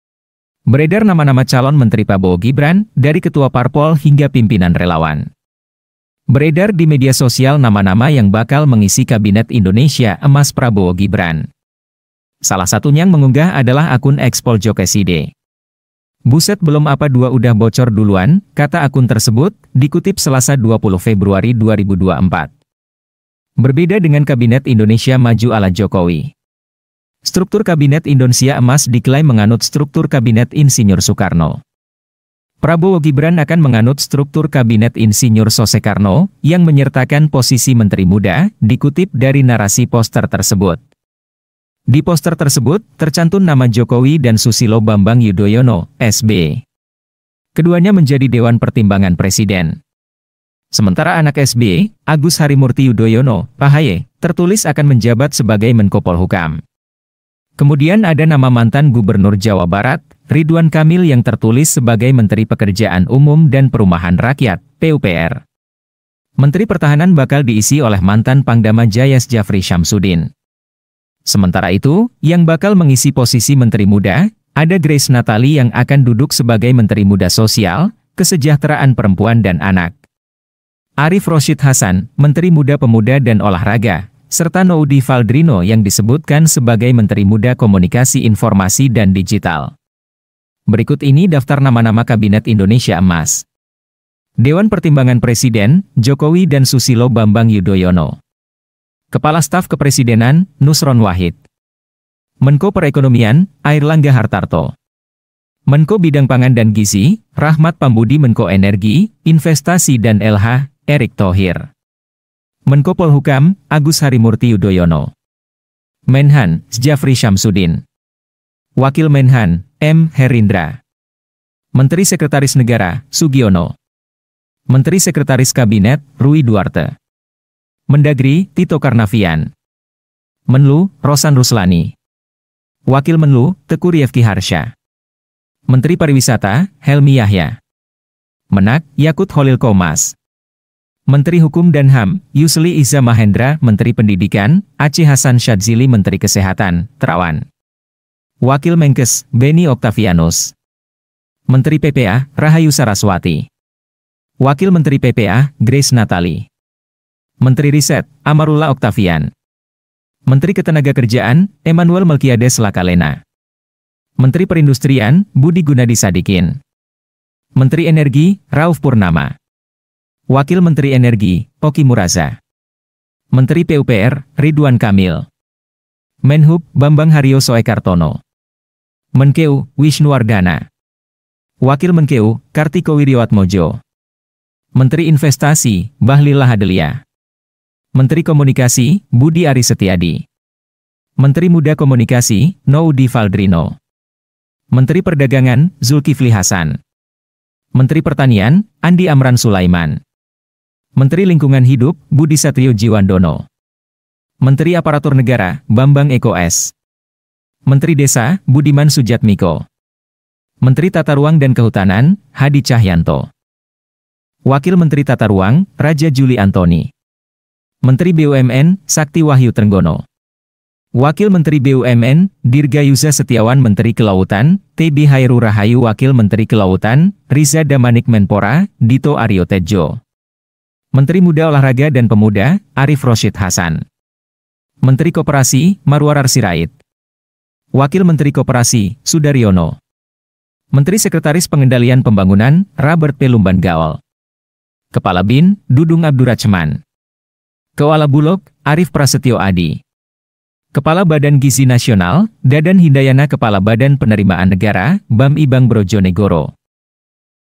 Beredar Nama-nama Calon Menteri Prabowo-Gibran, dari Ketua Parpol hingga Pimpinan Relawan
"Buset belom apa2 udah bocor duluan," kata akun tersebut, dikutip Selasa 20 Februari 2024.

Berbeda dengan Kabinet Indonesia Maju ala Jokowi. Struktur Kabinet Indonsia Emas diklaim menganut struktur kabinet Ir Soekarno.